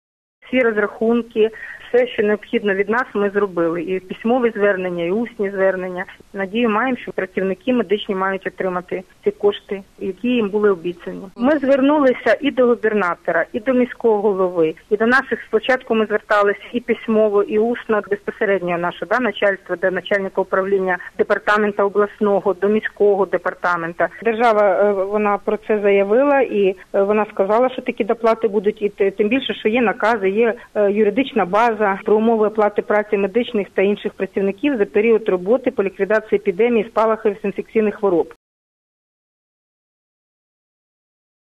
У Хмельницькій інфекційні лікарні для лікування хворих на COVID-19 задіяно понад 100 працівників
У Хмельницькій інфекційні лікарні для лікування хворих з коронавірусом  задіяно понад 100 працівників, що складає ⅔ від усього штату. Заклад є базовим для лікування хворих з COVID-19 в області.  Про це Суспільному розповіла головна лікарка Хмельницької міської інфекційної лікарні Оксана Піддубна. За її словами аби отримати анонсові державо доплати за лікування хворих на covid-19, працівники Хмельницької інфекційної лікарні звернулися до голови облдержадміністрації  та міського голови Хмельницького.
Нагадаємо,30 березня 2020 року парламент ухвалив законопроект №3275, який встановлює надбавки у 300% до заробітної плати медичним та іншим працівникам, залученим до протидії епідемії коронавірусу.